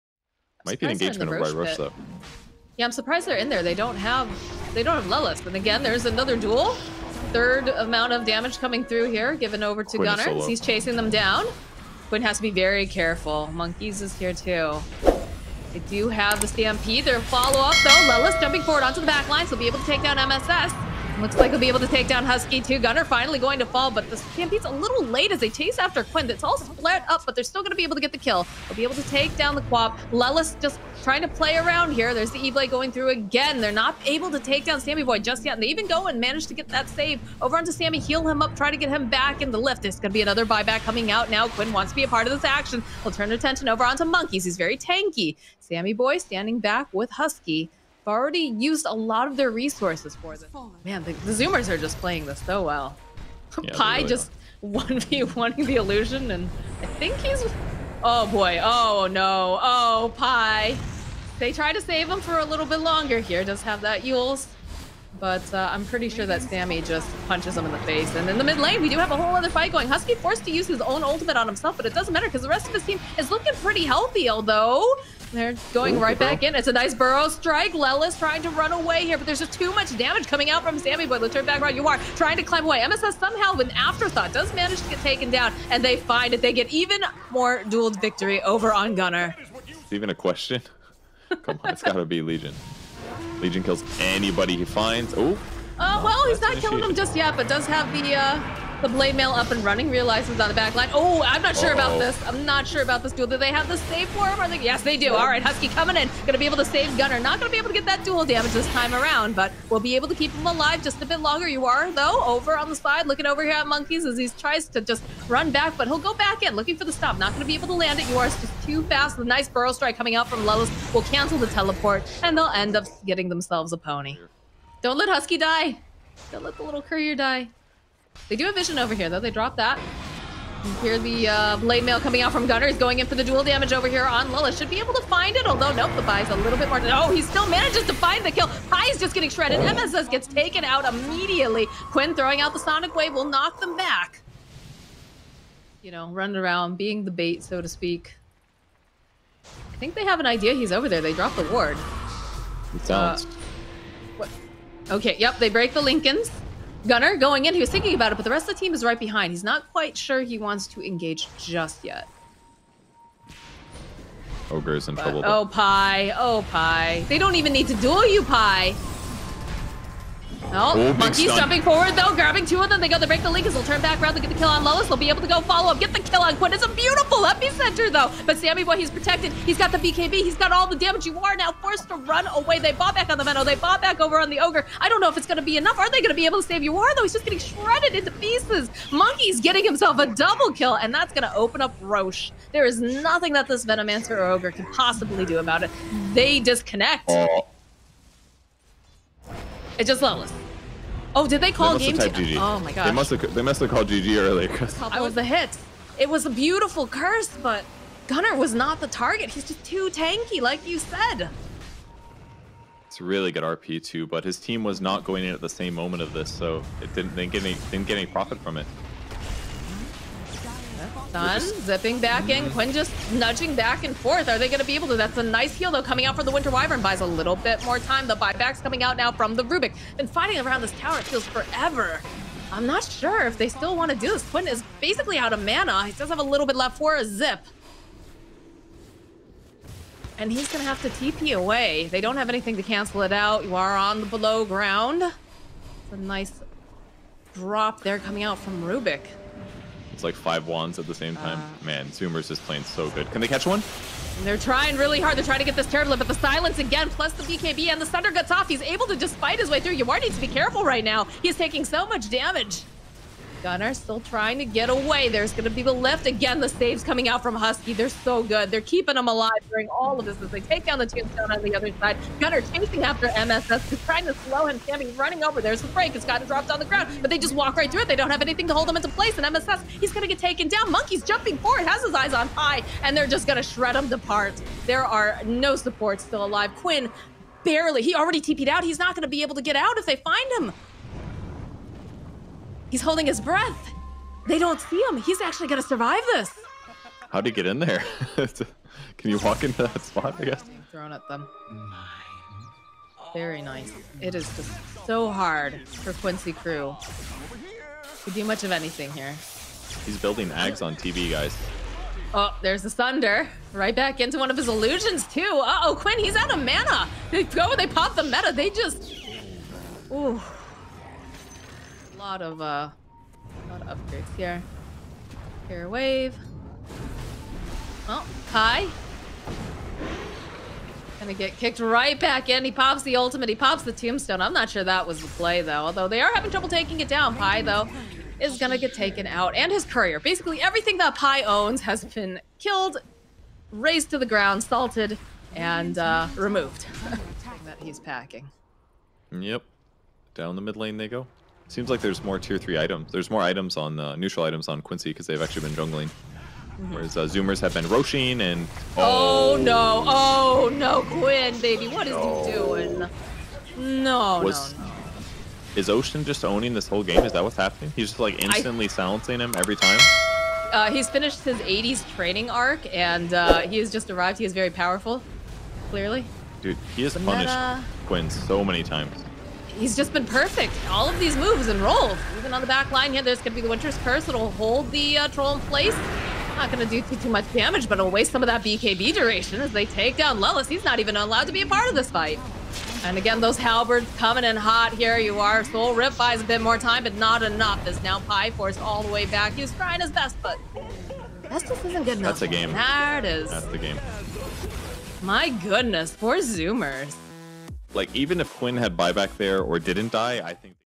Quincy Crew vs 4 Zoomers - ESL One Thailand 2020 America - Group Stage
Full game highlights of Quincy Crew vs 4 Zoomers at the ESL One Thailand 2020 America group stage.